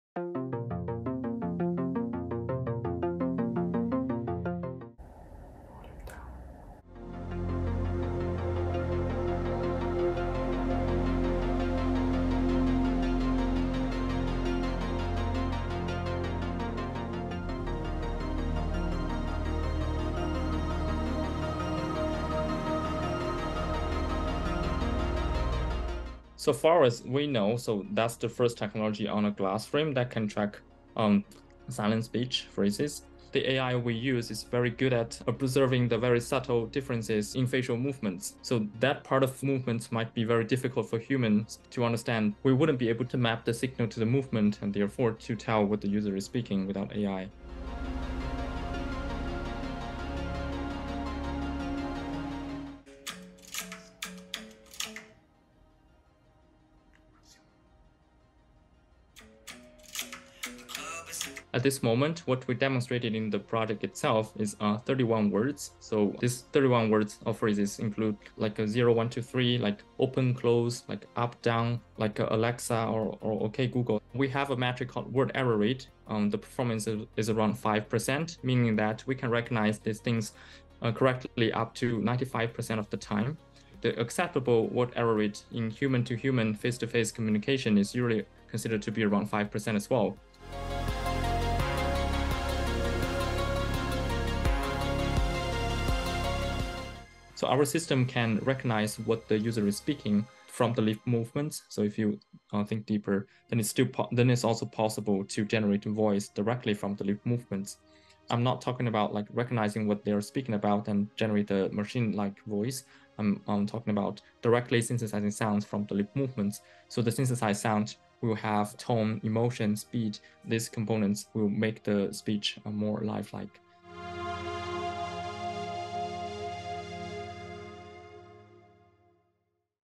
How AI can help people with disabilities
Researchers are finding in AI the technology essential for work that can significantly improve lives. One such project is called EchoSpeech, the first technology on a glass frame that can track silent speech phrases. Take a look.